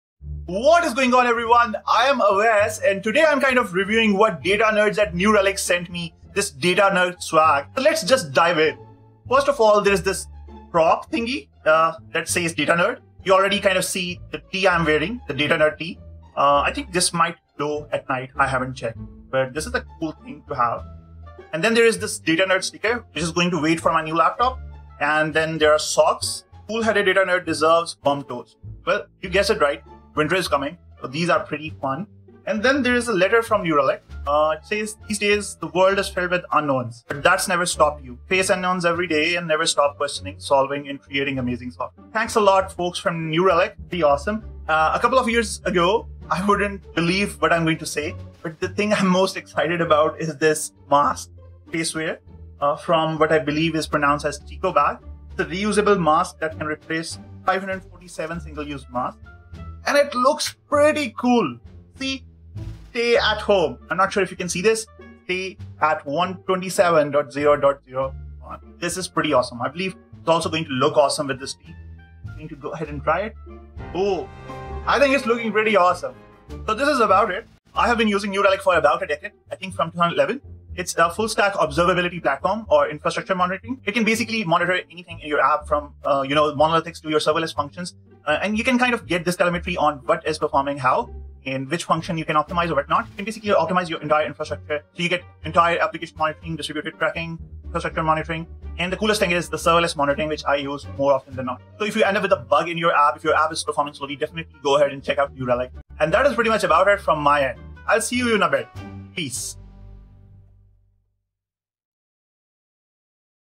New Relic Sent me DataNerd Swag | Unboxing
Awesome folks at sent me the DataNerd swag so I did my first ever unboxing video. I should probably do more of these. Share what you think of this and I'll see you in a bit.

Peace! ✌️